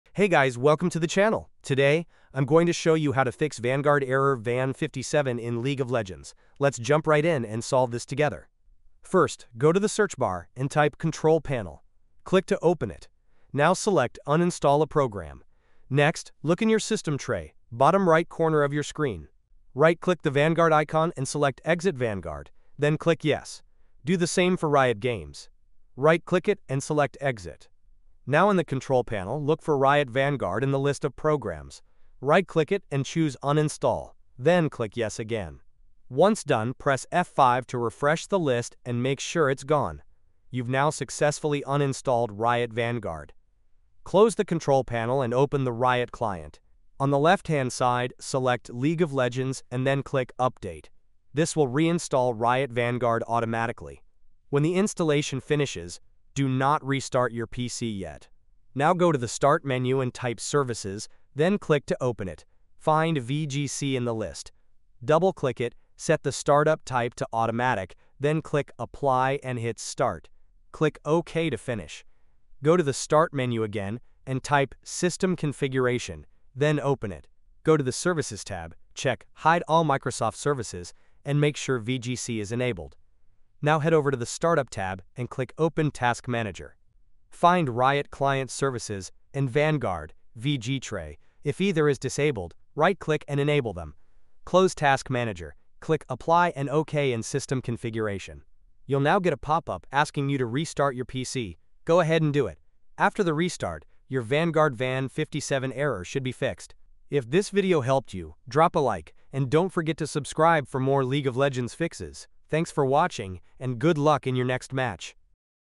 How to Fix Vanguard Error Van 57 in League of Legends - NEW 2026🎮 🔥
Getting the Vanguard VAN 57 error when trying to play League of Legends? This issue is caused by Riot’s anti-cheat system not starting properly, especially Riot Vanguard.

In this quick and easy guide, I’ll show you how to fix VAN 57 step-by-step, so you can get back into your matches without frustration!